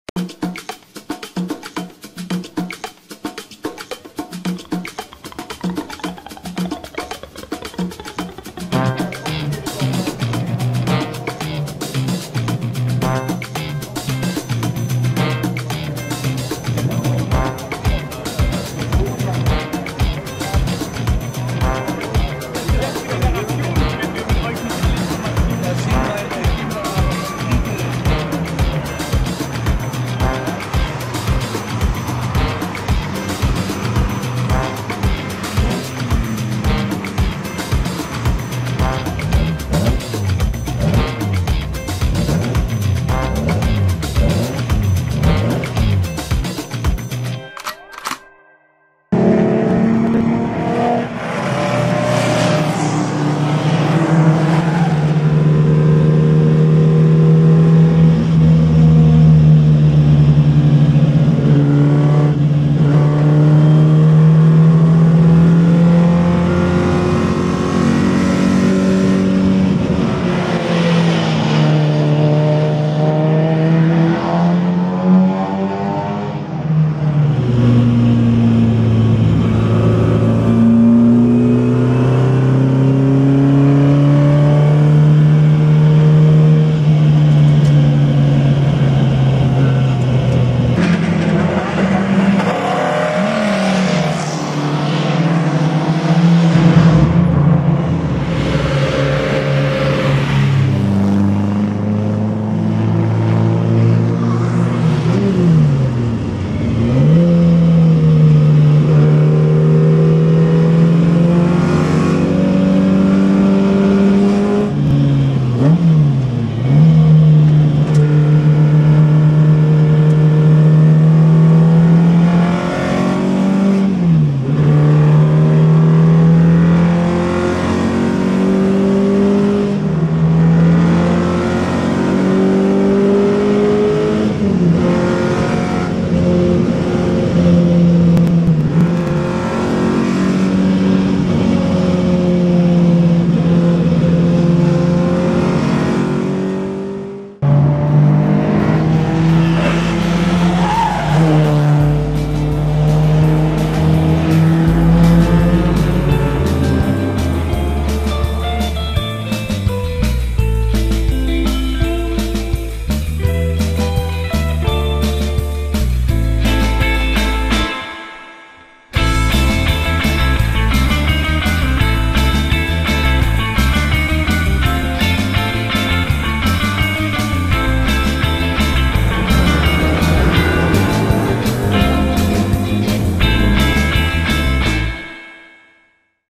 GTAm Spettacolo Sportivo Zandvoort 2013
OKP Racing Club has a little fun at Zandvoort